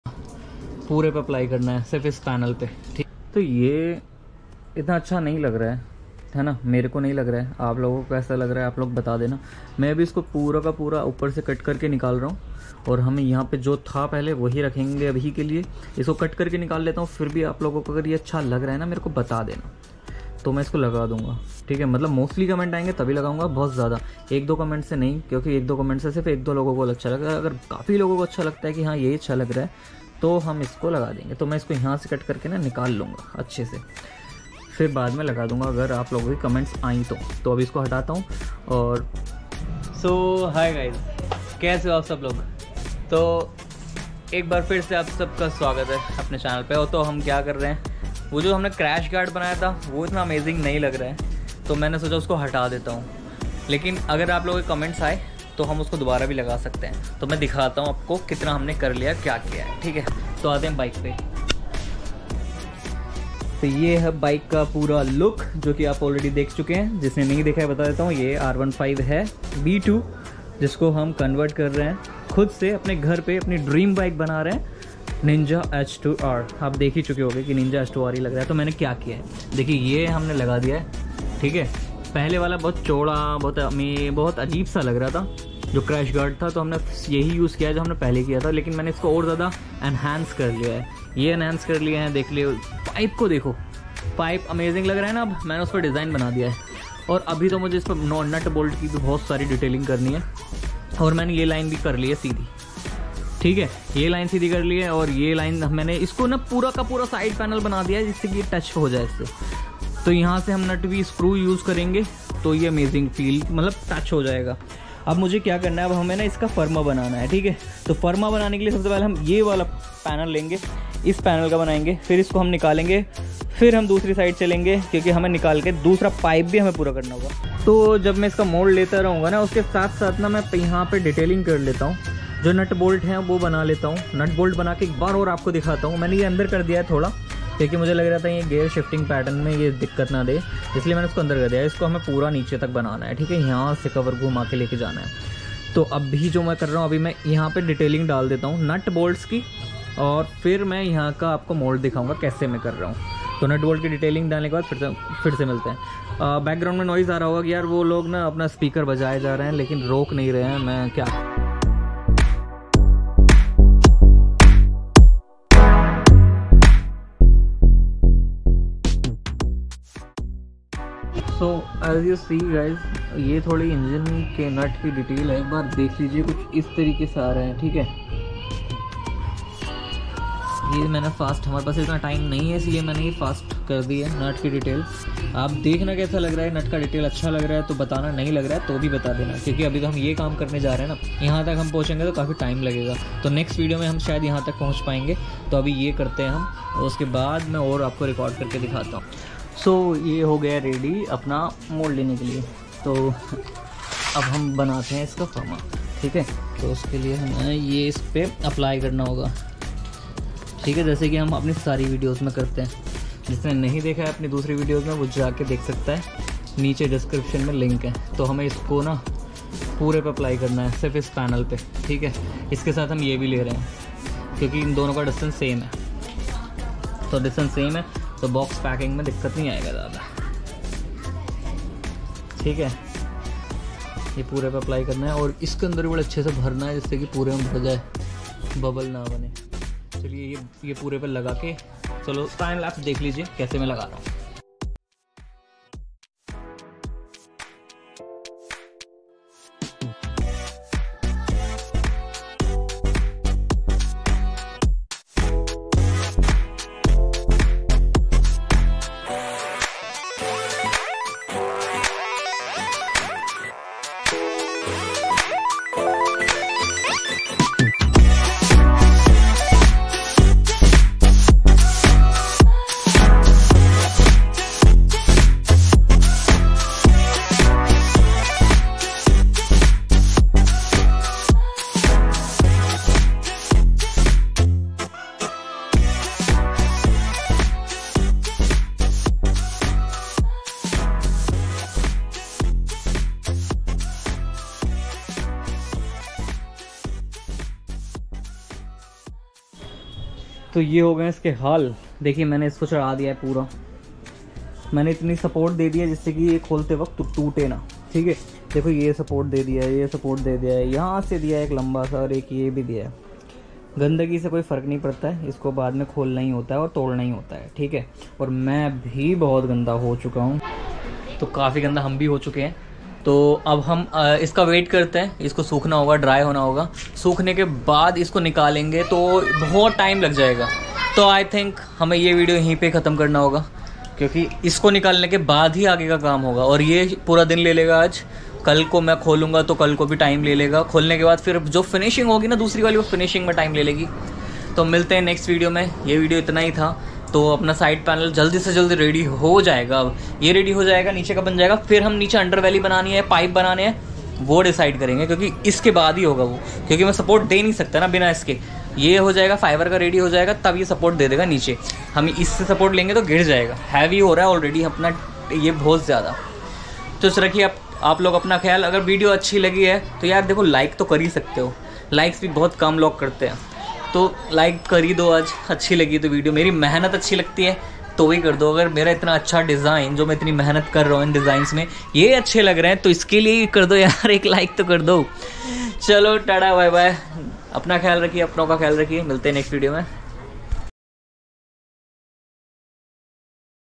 Ab jaldi ready ho jaegi Apni dream bike 😍|| Mould making start🔥| home made h2r from r15 v2
Finally side panel ka mould start || Ninja h2R ka side panel bn gya | home made h2r from r15 v2

Order body kits on - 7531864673

Ninja H2R project